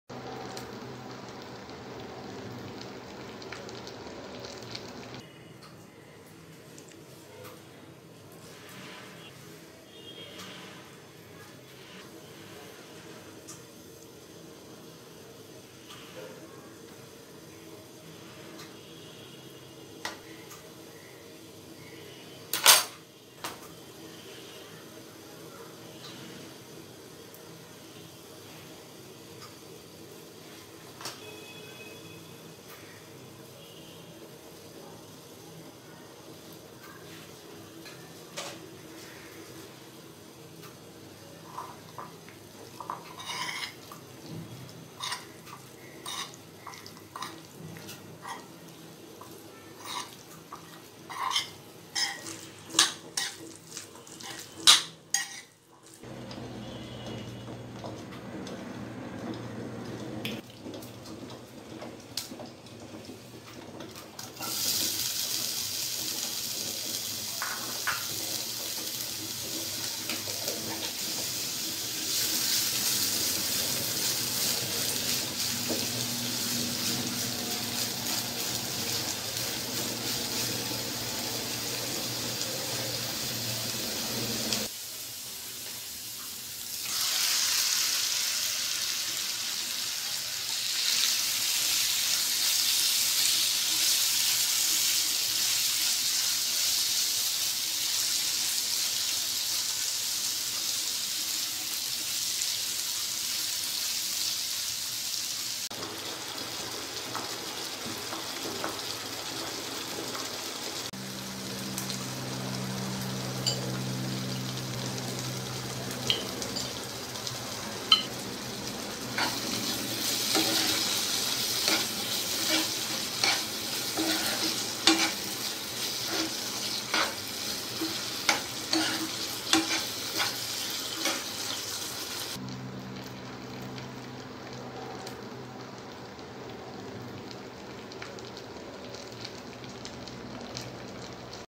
Chicken Curry Recipe | Bhuna Chicken | Chicken Kosha
ingredients-

boneless chicken-300g
onion paste-1 tsp
coriander powder-1
salt
mustard oil-2 tsp
oil
chopped onion-1/2 cup
bay leaves-2
garam masala-1 tsp
green chilli-3
coriander